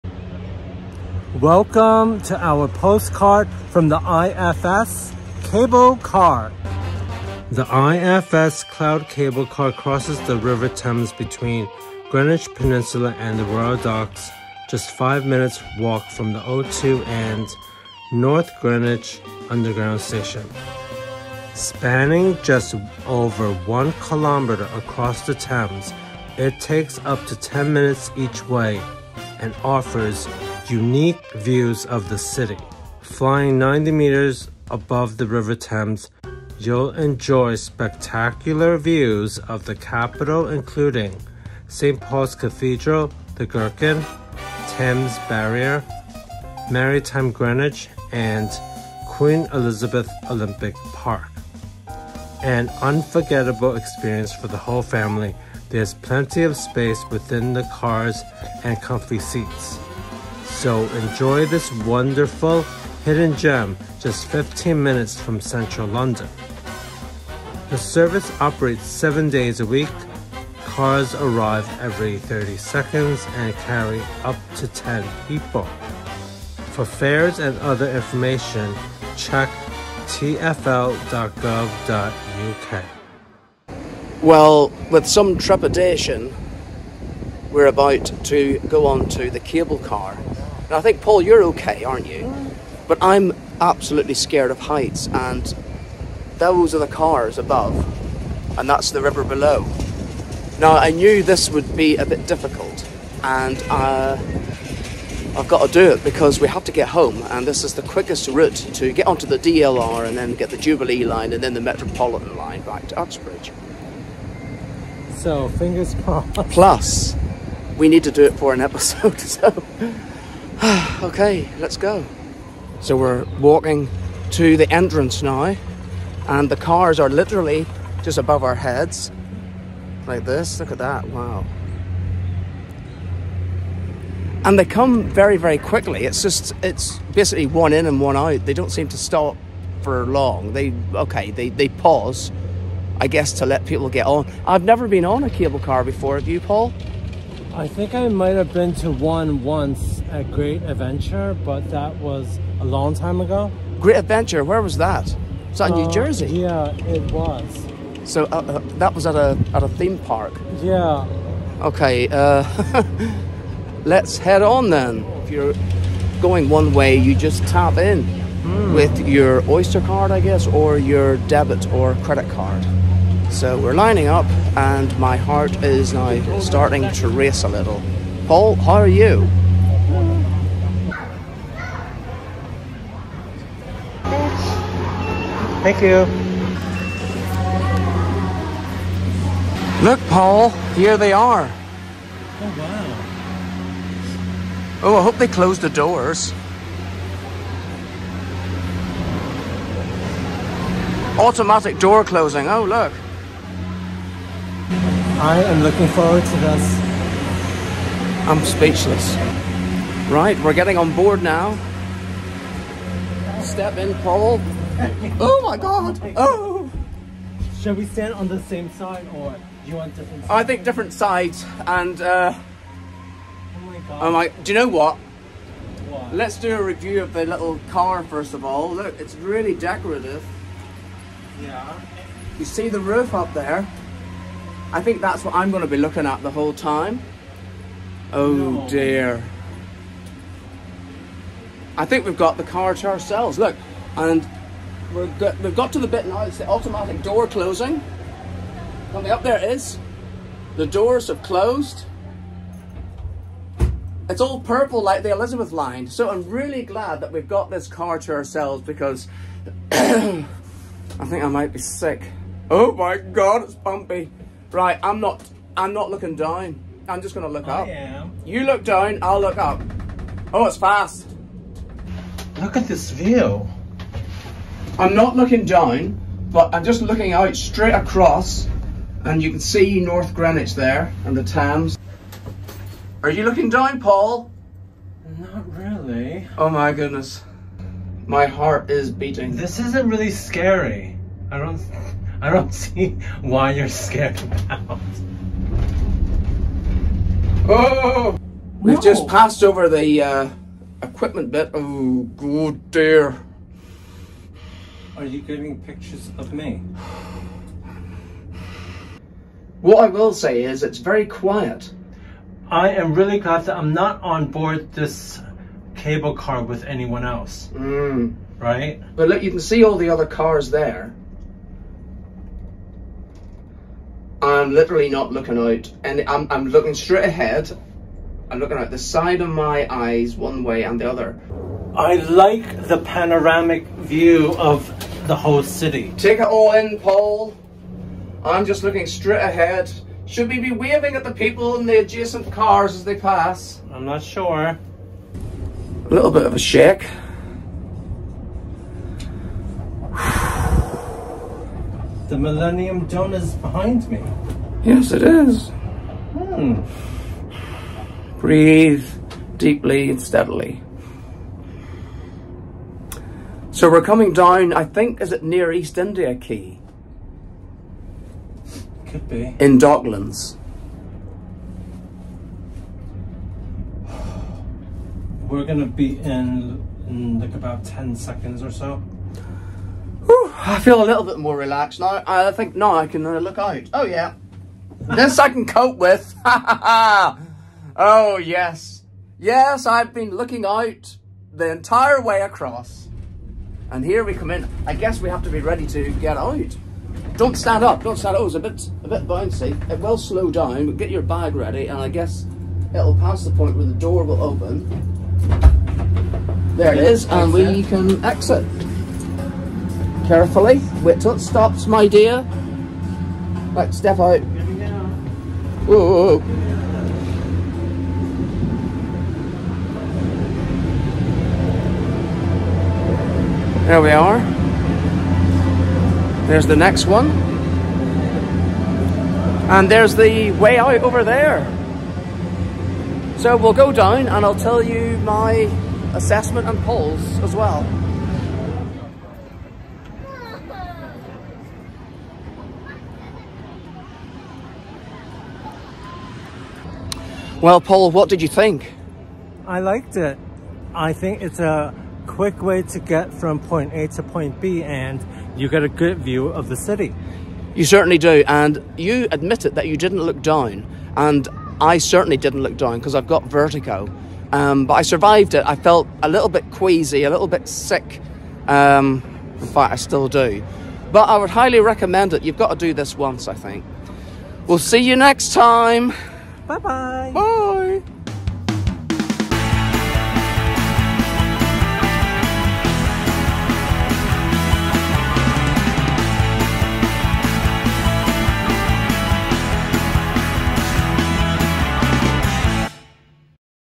Bald bloke who is scared of heights rides the IFS Cloud Cable Car
Paul and Marcus take a trip across the Thames on board the IFS Cloud Cable Car. But how will Marcus cope with his vertigo?